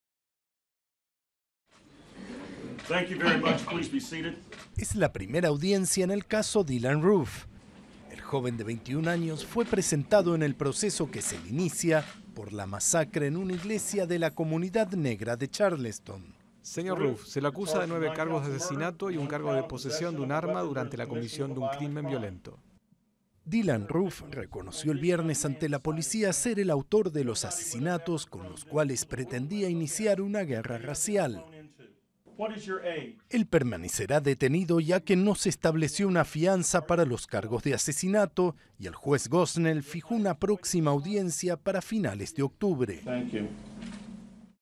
Sospechoso de masacre de Charleston frente al juez
Acusado de masacre de Charleston, en la que murieron nueve personas, confiesa que quería emprender una "guerra racial".